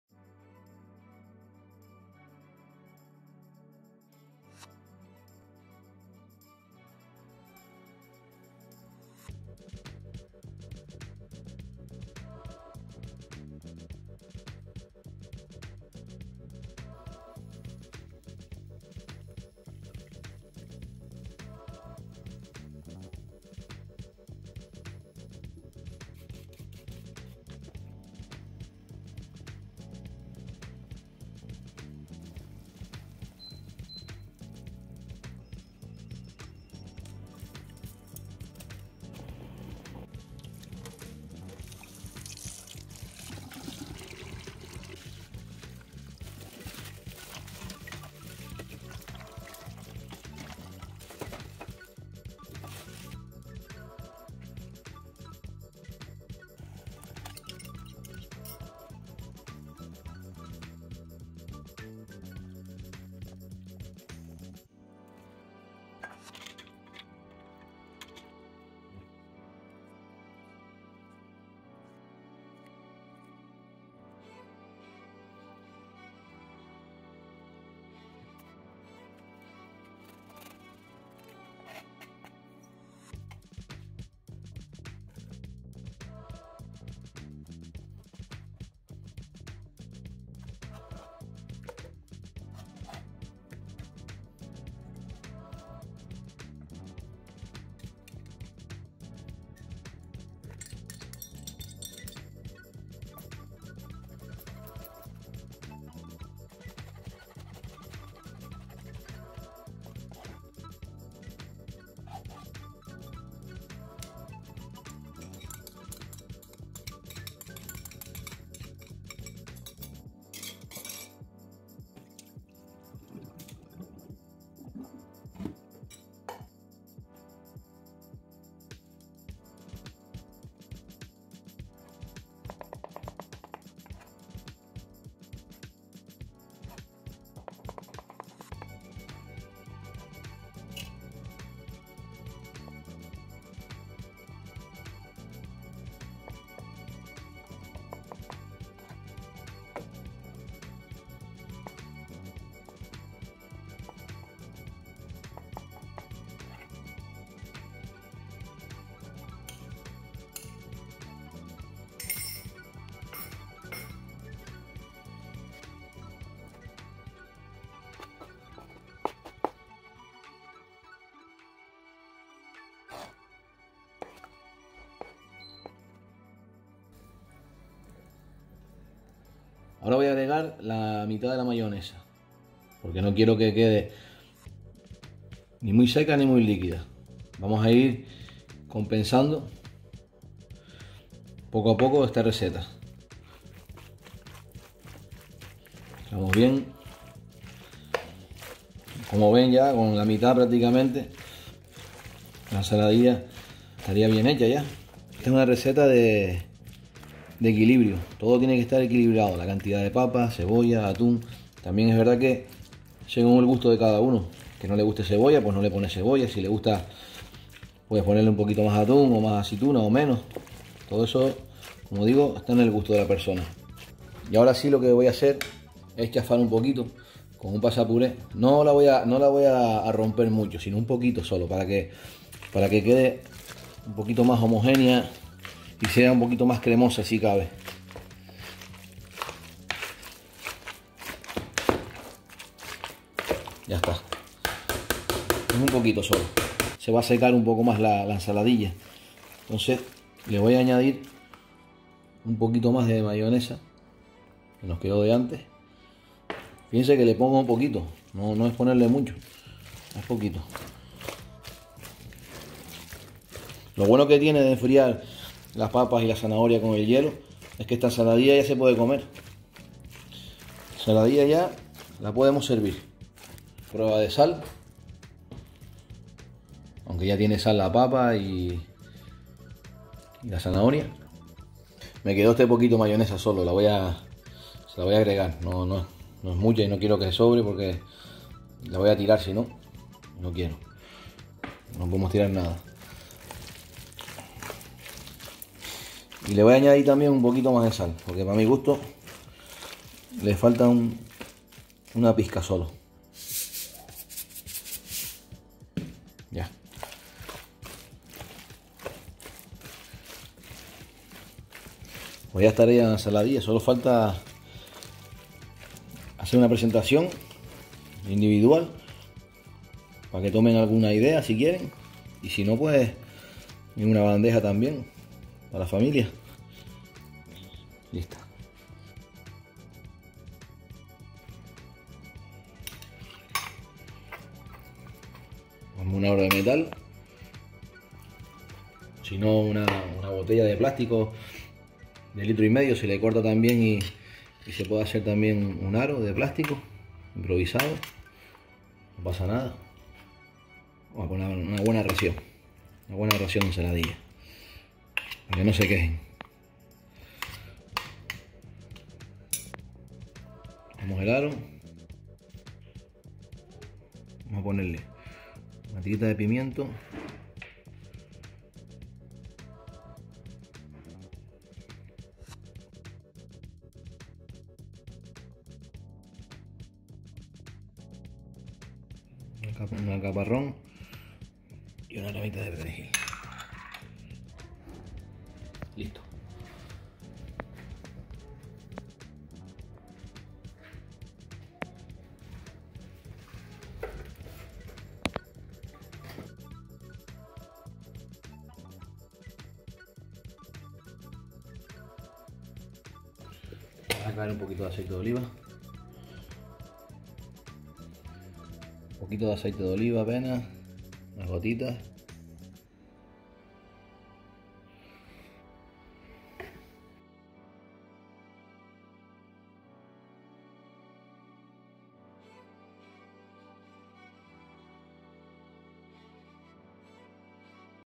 Ensaladilla de Atún y Langostinos 😋🥔🦐🐟➡️➡️➡️🍲
Ingredientes:

100 grs Aceitunas rellenas de anchoas
200 grs Langostinos pelados crudos
5 Pimientos asados del piquillo
1/2 Cebolla blanca cortada
Mayonesa 350 ml
Atún 250 grs
Perejil fresco
3 Zanahorias
4 Alcaparras
8 Papas